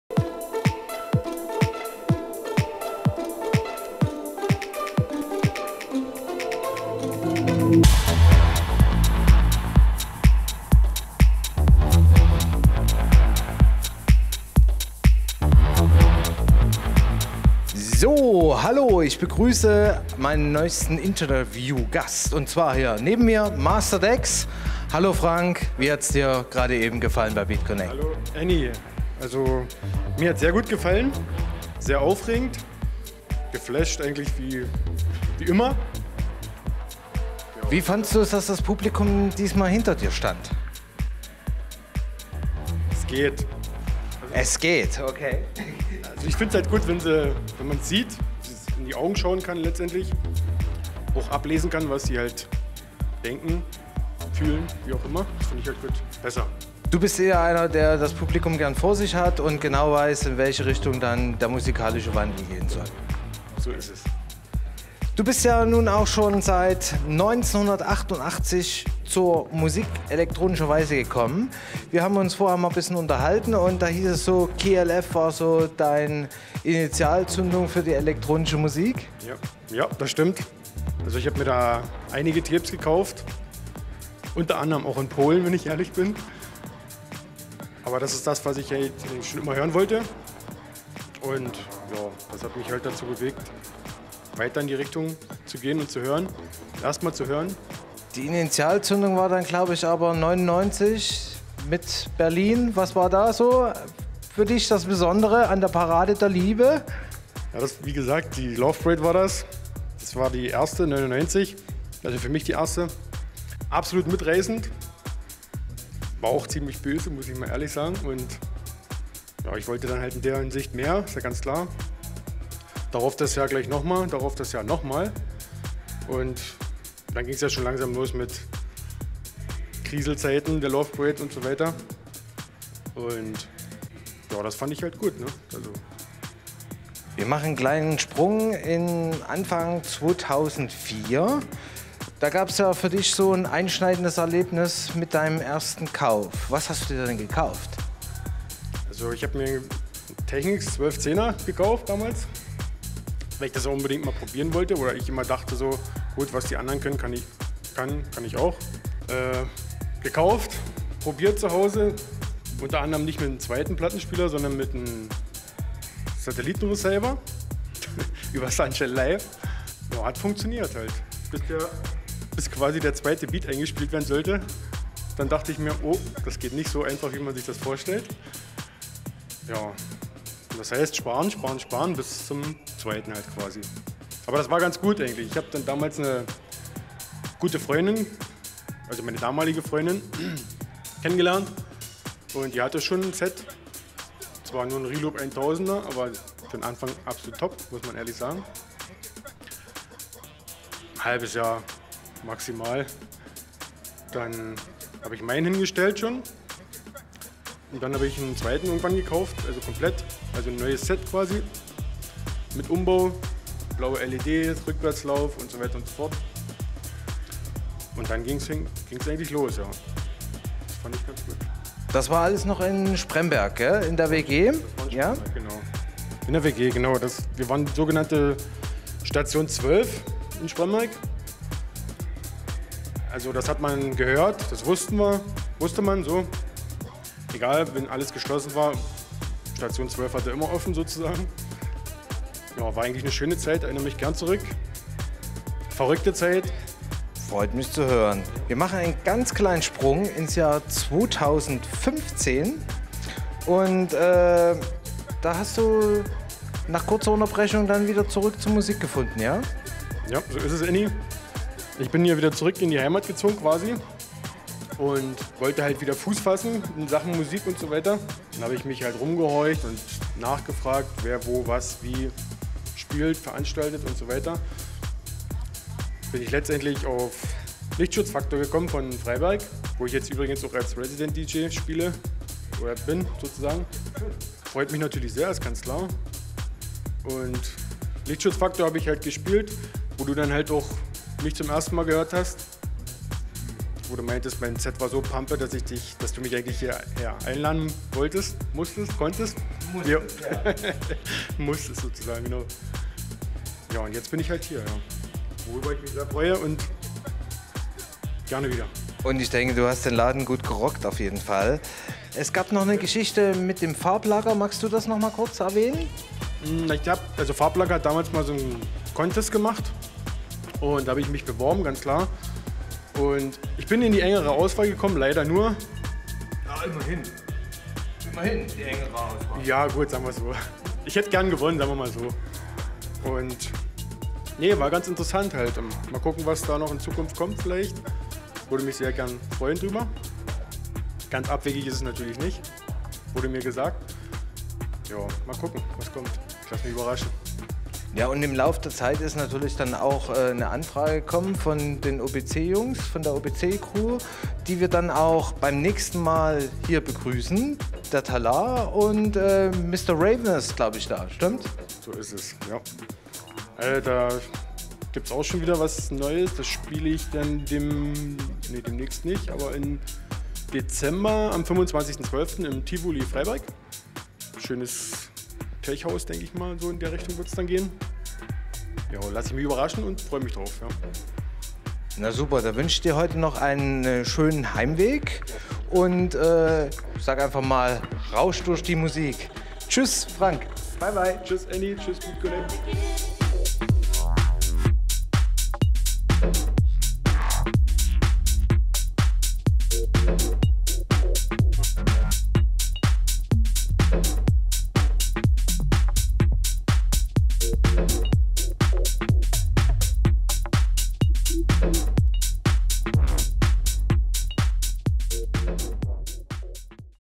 Beatconnect » Interview » MASTA DEX » 13.09.2017
Masta Dex mag dem tanzenden Publikum lieber ins Gesicht schauen und hat sich bei uns dennoch pudelwohl gefühlt. Kein Wunder, bringt er doch neben seiner Plattensammlung, jede Menge Routine an den Start.
Film: Framework Filmproduktion Chemnitz